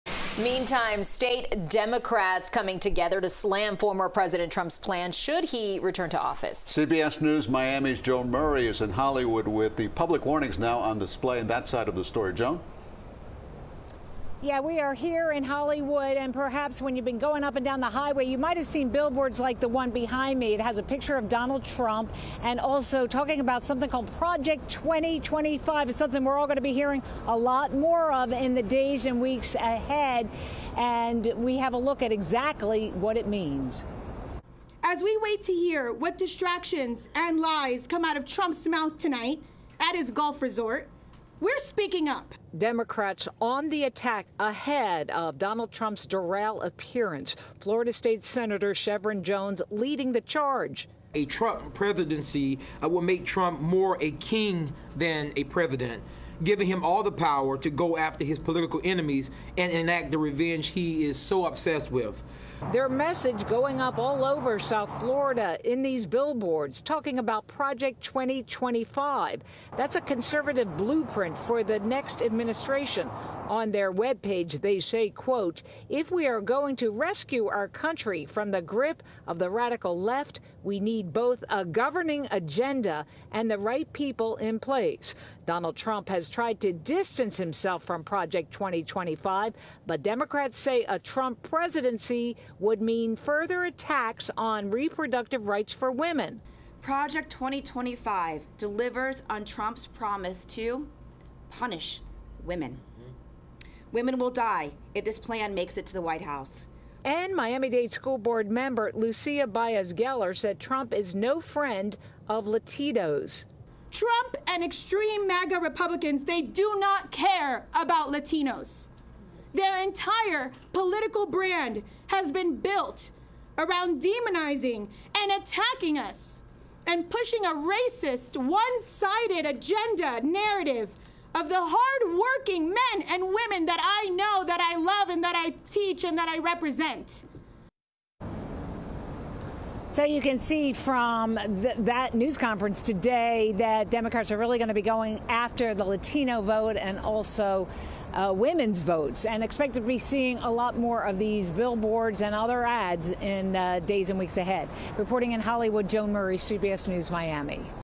FL Democrats slam Donald Trump on billboards
Democrats put up billboards about the dangers of a second presidency of Donald Trump and his Project 2025, a massive proposed overhaul of the federal government.